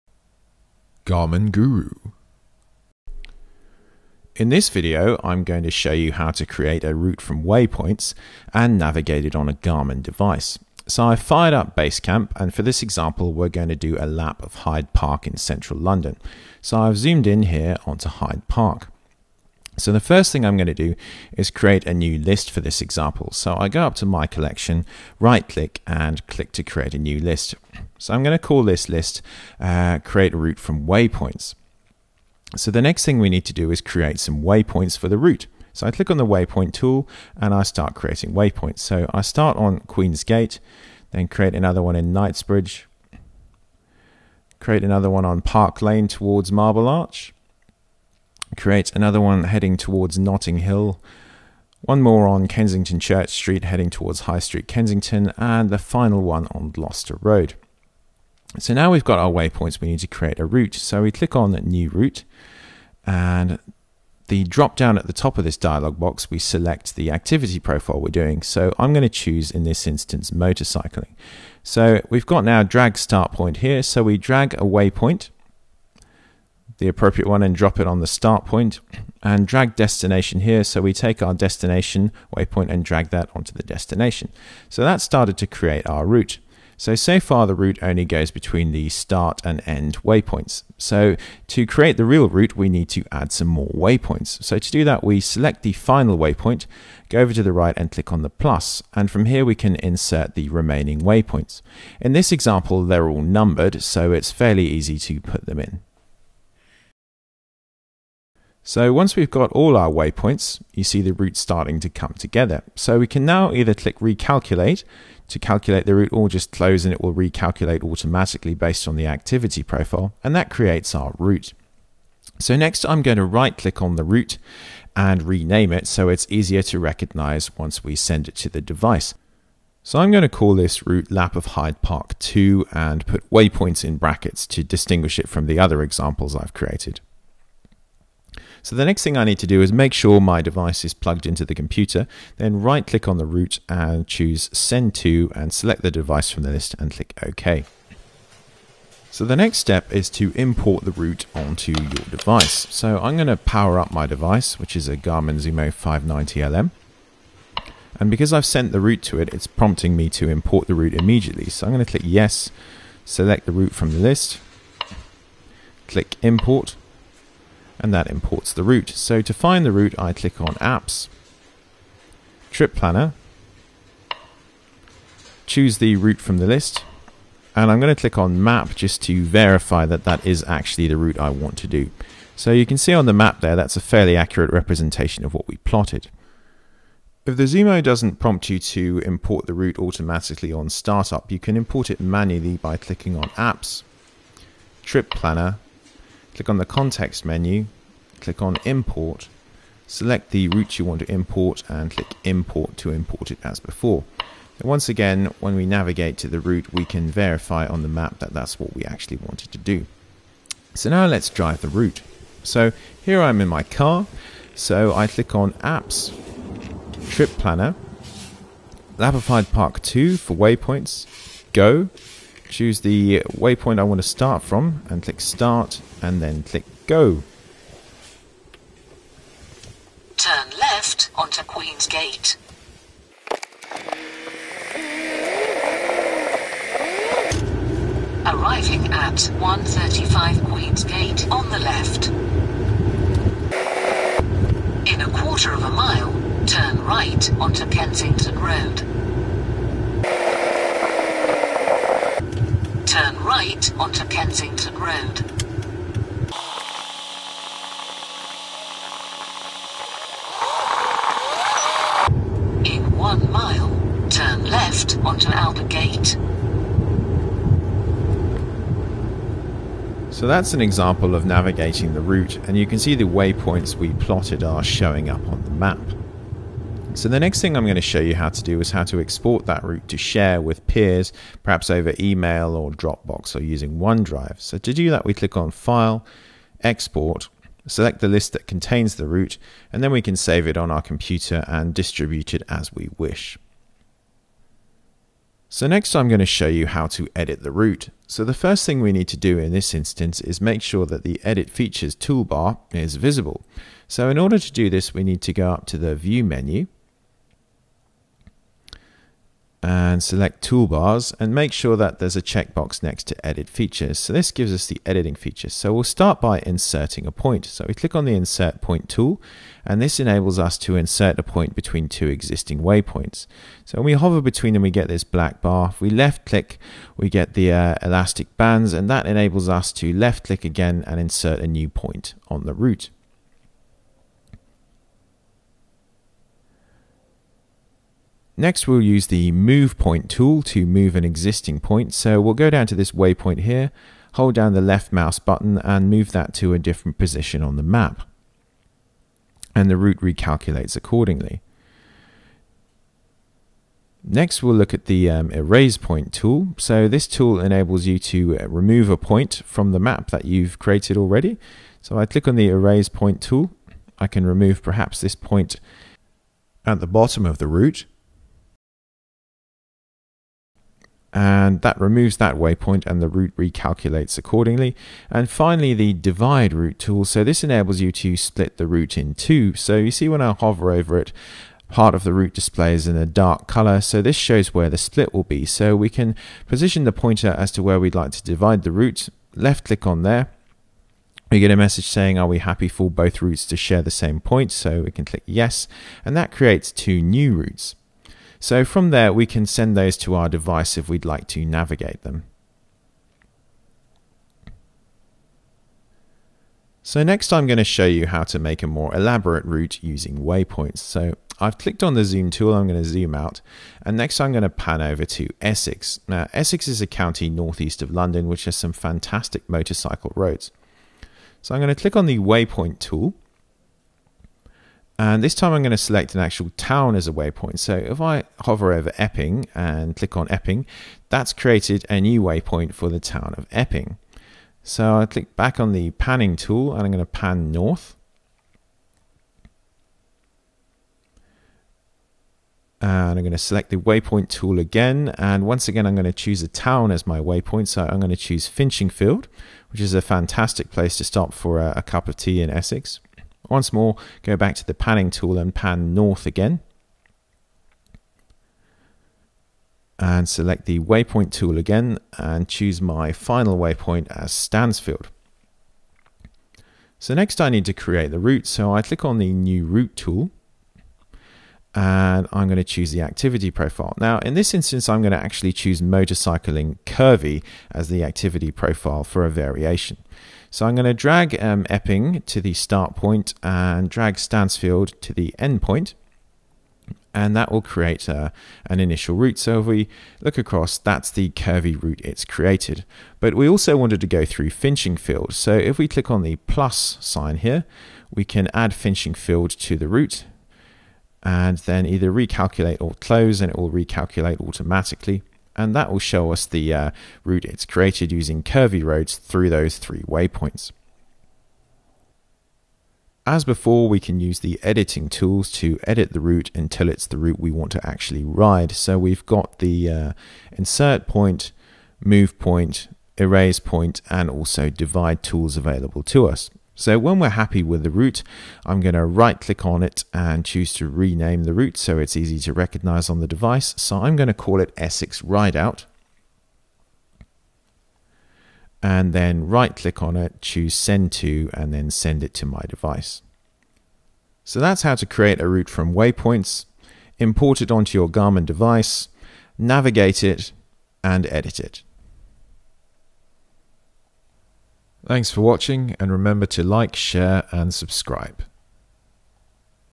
Garmin BaseCamp, How to create a route from Waypoints
This video describes how to create a route in BaseCamp from Waypoints and navigate it on a Garmin device.